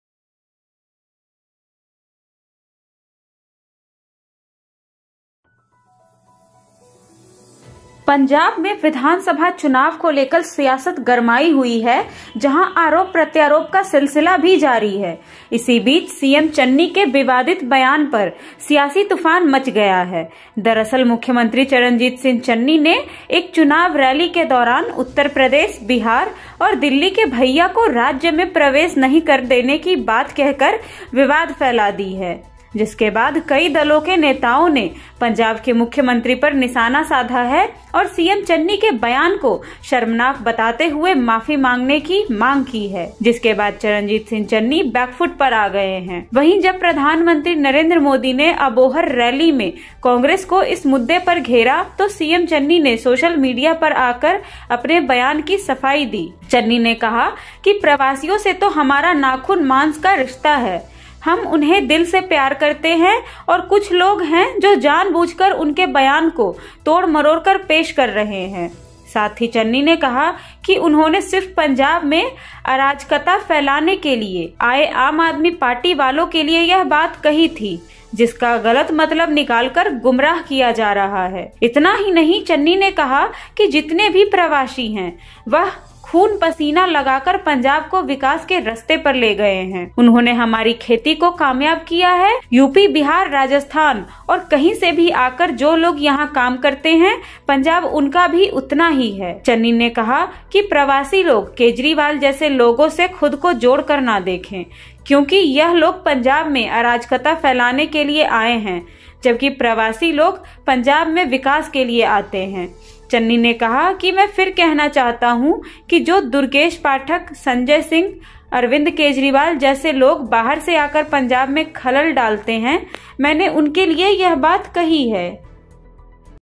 punjab विधानसभा चुनाव - UP-बिहार के भइये' वाले बयान के बाद CM चन्नी ने केजरीवाल पर किया पलटवार
पंजाब में विधान सभा चुनाव को लेकर सियासत गरमाई हुई हैं जहा आरोप प्रत्यारोप का सिलसिला जारी है इसी बीच सीएम चन्नी के विवादित बयान पर सियासी तूफान मच गया है. दरासल मुख्यमंत्री चरणजीत सिंह चन्नी ने एक चुनावी रैली के दौरान उत्तर प्रदेश, बिहार और दिल्ली के 'भैया' को राज्य में प्रवेश नहीं करने देने की बात कहकर विवाद फैला दी है जिसके बाद   कई दलों के नेताओं ने पंजाब के मुख्यमंत्री पर निशाना साधा है और सीएम चन्नी के बयान को शर्मनाक बताते हुए माफी मांगने की मांग की है.जिसके बाद चरण जीत सिंह चन्नी बैकफुट पर आ गए है

वही जब  प्रधानमंत्री नरेंद्र मोदी ने अबोहर रैली में कांग्रेस को इस मुद्दे पर घेरा तो सीएम चन्नी ने सोशल मीडिया पर आकर अपने बयान पर सफाई दी।

चन्नी ने कहा कि प्रवासियों से तो हमारा नाखून-मांस का रिश्ता है। हम उन्हें दिल से प्यार करते हैं और कुछ लोग है जो जानबूझकर उनके बयान को तोड़-मरोड़कर पेश कर रहे हैं साथ ही  चन्नी ने कहा कि उन्होंने सिर्फ पंजाब में अराजकता फैलाने के लिए आए आम आदमी पार्टी वालों के लिए यह बात कही थी। जिसका गलत मतलब निकालकर गुमराह किया जा रहा है।
इतना ही नही चन्नी ने कहा कि जितने भी प्रवासी हैं, वह खून-पसीना लगाकर पंजाब को विकास के रास्ते पर ले गए हैं। उन्होंने हमारी खेती को कामयाब किया है। यूपी, बिहार, राजस्थान और कहीं से भी आकर जो लोग यहां काम करते हैं, पंजाब उनका भी उतना ही है,
चन्नी ने कहा कि प्रवासी लोग केजरीवाल जैसे लोगों से खुद को जोड़कर न देखें। क्योंकि यह लोग पंजाब में अराजकता फैलाने के लिए आए हैं। जबकि प्रवासी लोग पंजाब में विकास के लिए आते हैं। चन्नी ने कहा कि मैं फिर कहना चाहता हूं कि जो दुर्गेश पाठक, संजय सिंह, अरविंद केजरीवाल जैसे लोग बाहर से आकर पंजाब में खलल डालते हैं, मैंने उनके लिए यह बात कही है।
'UP-बिहार के भइये' वाले बयान के बाद पंजाब के CM चरणजीत चन्नी ने केजरीवाल पर किया पलटवार